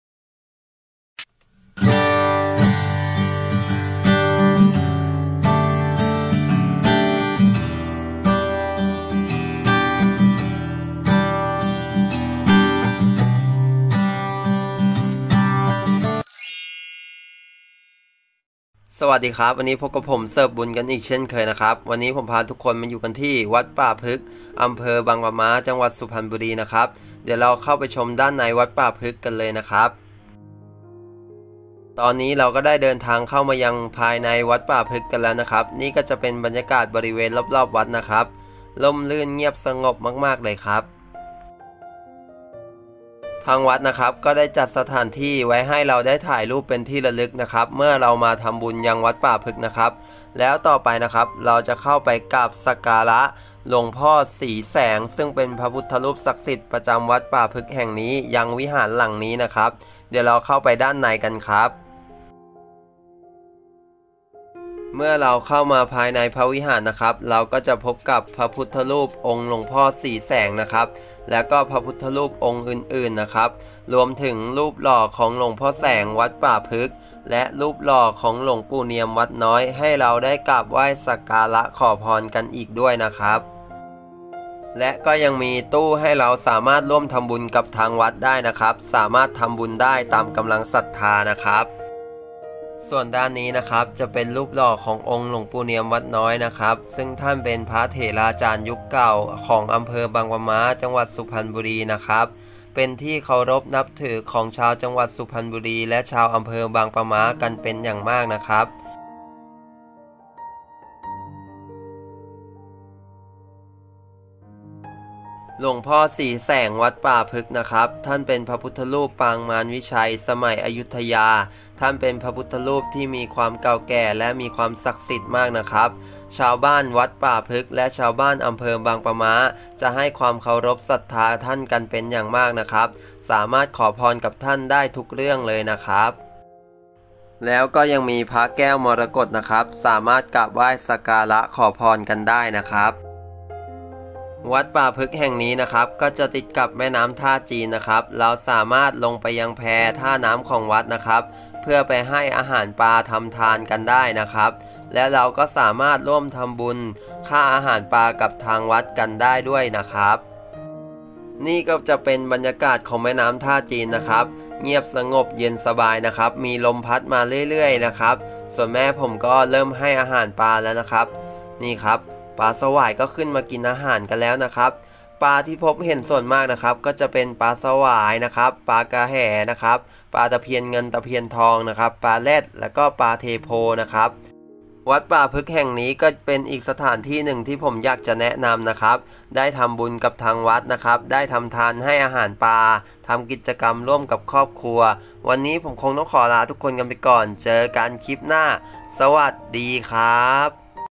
EP.53 วัดป่าพฤกษ์ อ.บางปลาม้า จ.สุพรรณบุรี กราบสักการะขอพรพระพุทธรูปศักดิ์สิทธิ์ หลวงพ่อสีแสง
สวัสดีครับ 🙏 วันนี้ผมพาทุกมาทำบุญกันที่วัดป่าพฤกษ์ อ.บางปลาม้า จ.สุพรรณบุรี กราบสักการะขอพรพระพุทธรูปศักดิ์สิทธิ์หลวงพ่อสีแสง ทำทานให้อาหารปลากันครับ